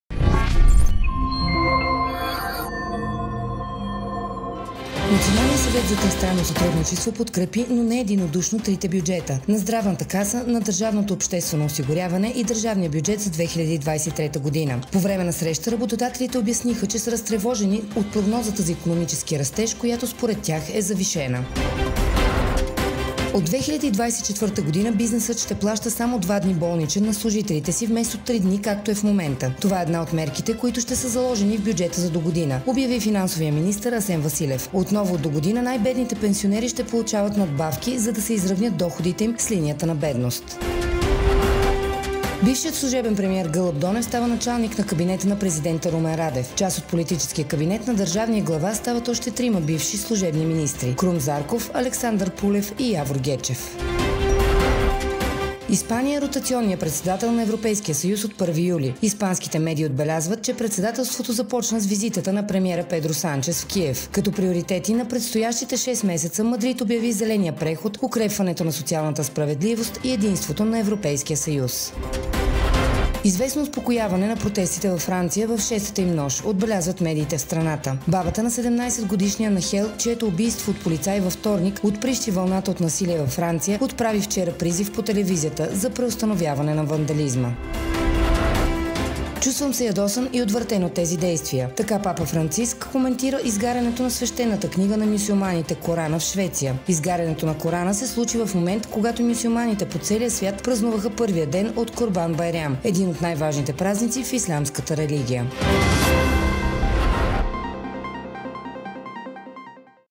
Breaking News 24/7: НОВИНИТЕ В 90 СЕКУНДИ НА 03 ЮЛИ
ТРИСТРАНКА

Националният съвет за тристранно сътрудничество подкрепи, но не единодушно трите бюджета - на НЗОК, на ДОО и държавния бюджет за 2023 г. По време на извънредната среща работодателите обясниха, че са разтревожени от прогнозата за икономическия растеж, която според тях е завишена.

БЮДЖЕТ

От 2024 г. Бизнесът ще плаща само два дни болничен на служителите си, вместо три дни както е в момента. Това е една от мерките, които ще са заложени в бюджета за догодина, обяви финансовият министър Асен Василев. Отново от догодина най-бедните пенсионери ще получават надбавки, за да се изравнят доходите им с линията на бедност.

ПОЛИТИЧЕСКИ КАБИНЕТ

Бившият служебен премиер Гълъб Донев става началник на кабинета на президента Румен Радев. Част от политическия кабинет на държавния глава стават още трима бивши служебни министри – Крум Зарков, Александър Пулев и Явор Гечев.

ПРЕДСЕДАТЕЛСТВО НА ЕС

Испания е ротационния председател на ЕС от 1 юли. Испанските медии отбелязват, че председателството започна с визитата на премиера Педро Санчес в Киев.  Като приоритети на предстоищите 6 месеца Мадрид обяви зеления преход, укрепването на социалната справедливост и единството на ЕС.

ПАРИЖ

Известно успокояване на протестите във Франция във шестата им нощ, отбелязват медиите в страната.

Бабата на 17-годишния Нахел, чието убийство от полицай във вторник отприщи вълната от насилие във Франция, отправи вчера призив по телевизията за преустановяване на вандализма.

КОМЕНТАР

"Чувствам се ядосан и отвратен от тези действия". Така Папа Франциск коментира изгарянето на свещената книга на мюсюлманите - Корана в Швеция. Изгарянето на Корана се случи в момент, когато мюсюлманите по целия свят празнуваха първия ден от Курбан Байрам, един от най-важните празници в ислямската религия.